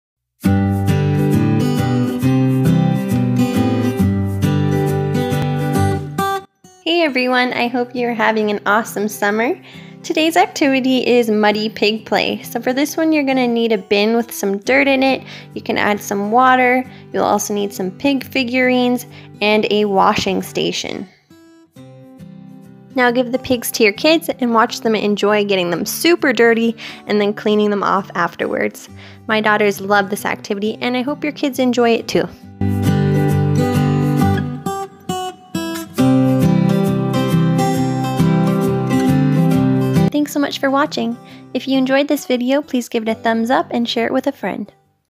Muddy Pigs Fun Activity
A fun activity for the backyard - muddy pigs play plus washing station!

Add dirt, water, and pig toys to the bin. Let your kids dive in with their hands and get the pigs super messy!! Bring the dirty pigs over to the washing station for a bath. Rinse and repeat for continued fun! :)